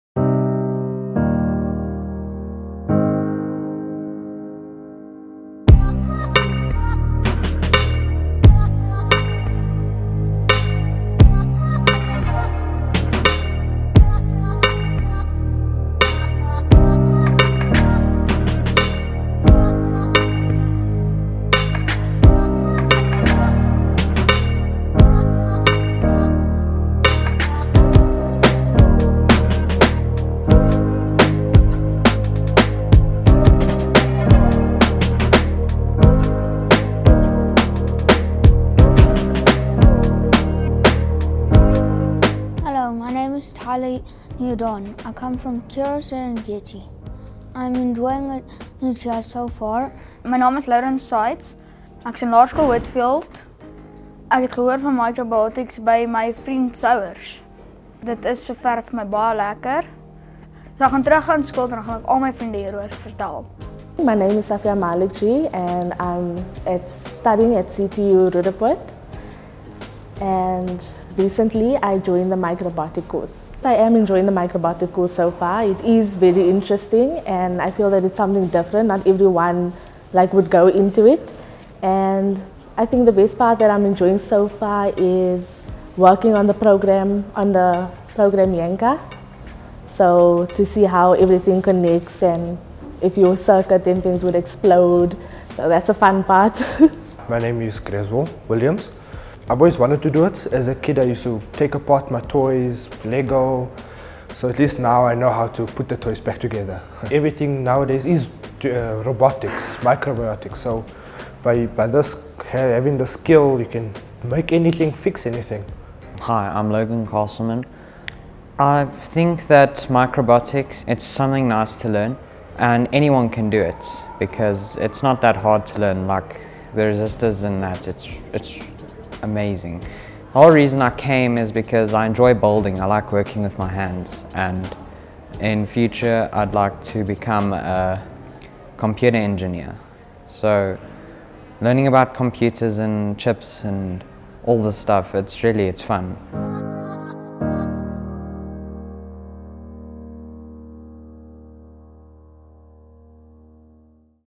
CTU Microbotics Part 1
What is Microbotics?

In a growing world that continually adopts modern technologies and devices, we envision a future of young digital makers, not only digital consumers! Microbotics is all about creativity and having fun.